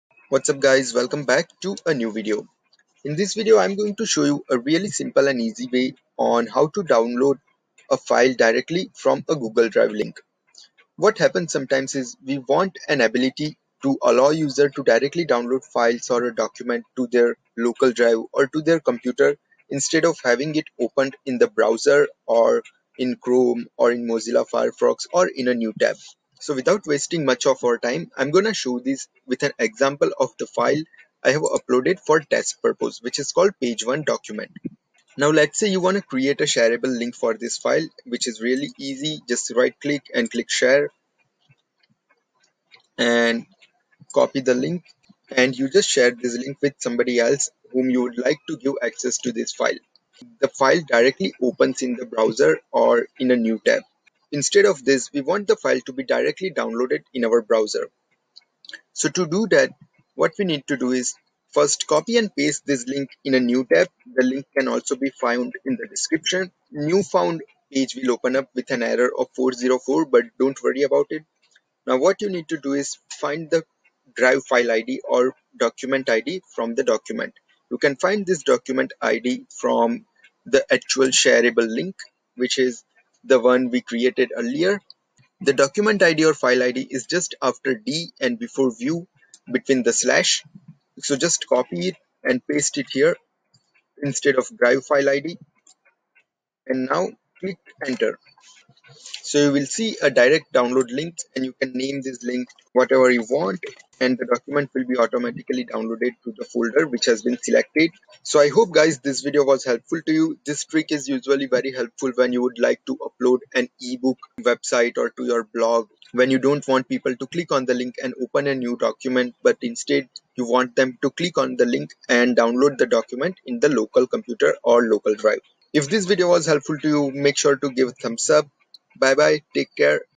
Directly Download Google Files instead of opening it in the GDrive (Updated 2021)
Do you want to create a direct download link for Google Drive Docs or download files from Google Drive? Here's a step-by-step instructional video on how to Directly Download Google Files instead of opening it in the Gdrive. This is also the solution for questions like how to add direct download link on website or on blogger. Hope this helps.